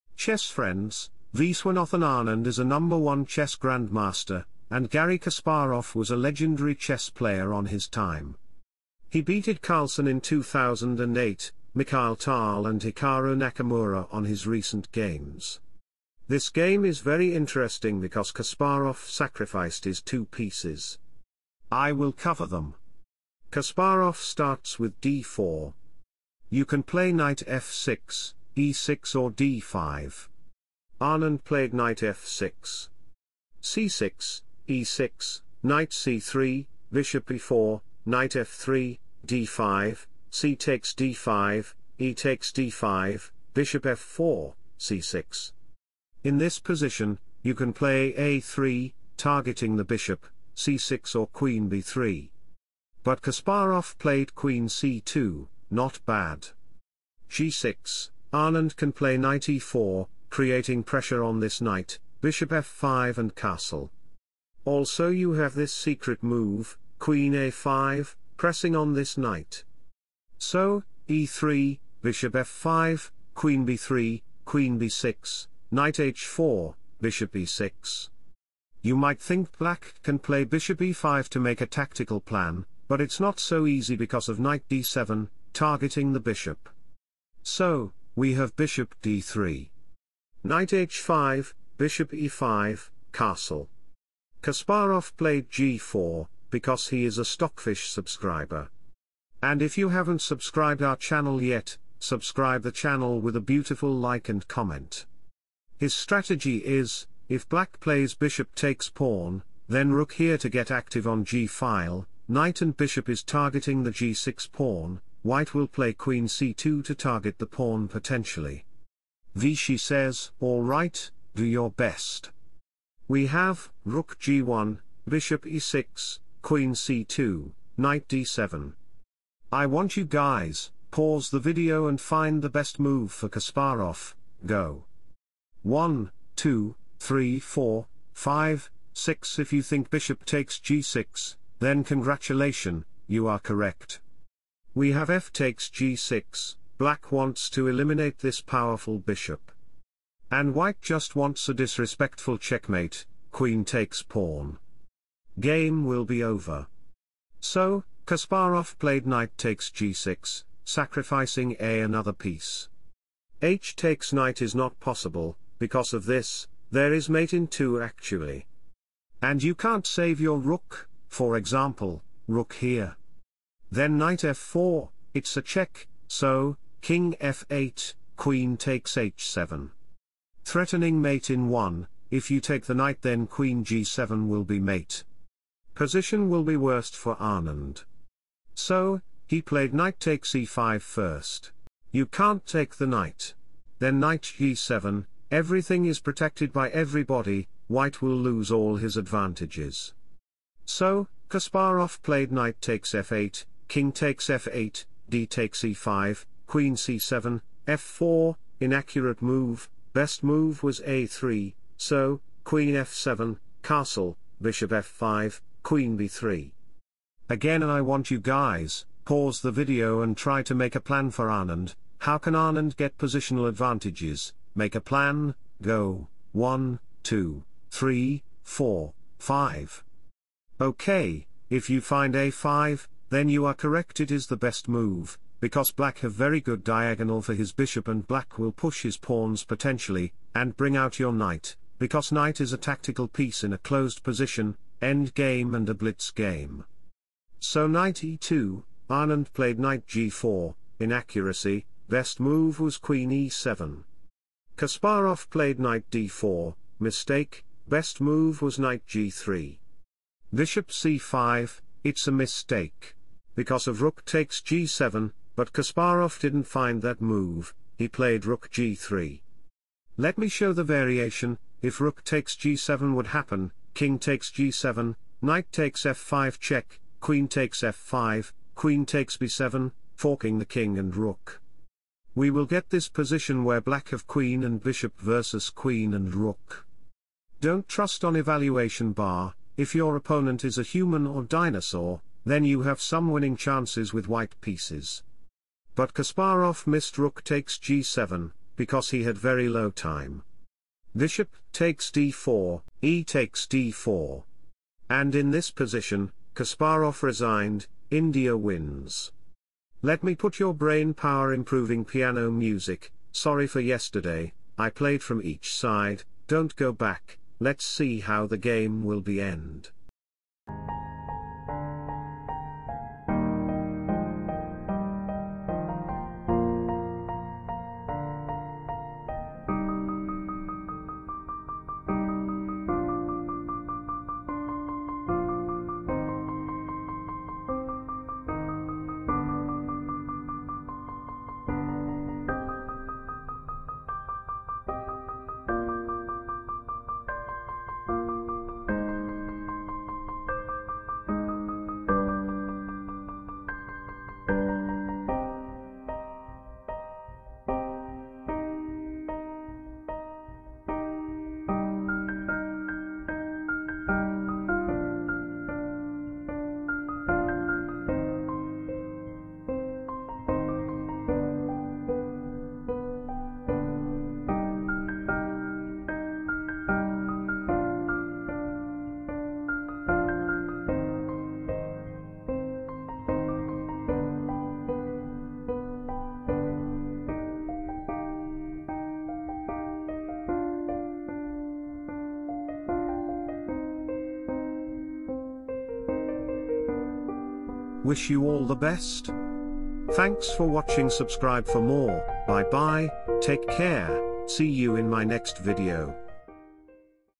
Garry Kasparov Sacrificed TWO PIECES But It's Vishy Anand who will win | Vishy vs kasparov 2021
Hi Chess Friends, Welcome to Ai chess channel. This video is about Viswanathan anand and garry kasparov 2021.

PGN-
[Event "Grand Chess Tour Croatia Rapid & Blitz 2021"]
[Site ""]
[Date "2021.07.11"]
[Round "?"]
[White "Garry Kasparov"]
[Black "Viswanathan Anand"]
[Result "0-1"]
1. d4 Nf6 2. c4 e6 3. Nc3 Bb4 4. Nf3 d5 5. cxd5 exd5 6. Bf4 c6 7. Qc2 g6 8. e3
Bf5 9. Qb3 Qb6 10. Nh4 Be6 11. Bd3 Nh5 12. Be5 O-O 13. g4 Bxg4 14. Rg1 Be6 15.
Qc2 Nd7 16. Bxg6 fxg6 17. Nxg6 Nxe5 18. Nxf8+ Kxf8 19. dxe5 Qc7 20. f4 Qf7 21.
O-O-O Bf5 22. Qb3 a5 23. Ne2 Ng7 24. Nd4 Bc5 25. Rg3 Bxd4 26. exd4
{Stockfish 15 : d=18} 26... a4 27. Qb4+ Kg8 28. e6 Qc7 29. Re1 Qxf4+ 30. Qd2
Qxd2+ 31. Kxd2 Be4 32. Rf1 Bg6 33. e7 Re8 34. Re3 Nf5 35. Re5 Nxd4 36. Rf4 Nb5
37. h4 Nd6 38. h5 Bf7 39. Kc1 h6 40. Rg4+ Kh7 41. Rf4 Kg7
{-+ (-2.36) Stockfish 15 : d=16} 42. Rxa4 Ne4 43. Rf5 Rxe7 44. b3 Nd6 45. Rg4+
Kh7 46. Rff4 Bxh5 47. Rg1 Re2 48. Rh1 Bg6 49. Rfh4 h5 50. Rxh5+ Bxh5 51. Rxh5+
Kg6 {-+ (-4.33) Stockfish 15 : d=17} 52. Rh8 Rxa2 53. Kb1 Rf2 54. Kc1 Kf7 55.
Rh6 Ke7 56. b4 Rf4 57. b5 Rc4+ 58. Kb2 cxb5 59. Rh7+ Ke6 60. Rh6+ Kd7 61. Rh8 d4
62. Rh3 Kc6 63. Kb1 {-+ (-6.78) Stockfish 15 :} 63... Rc3 64. Rh4 d3 65. Kb2 d2
66. Rd4 b4 67. Rxb4 d1=Q 68. Kxc3 Qe1+ 69. Kb3 Kc5 70. Ra4 Qd1+ 71. Kc3 Qxa4 72.
Kd3 Qe4+ 73. Kd2 Nc4+ 74. Kd1 b5 75. Kc1 Qe2 76. Kb1 Qb2# {Mate in -14} 0-1

2) Follow The Rule number 1 😉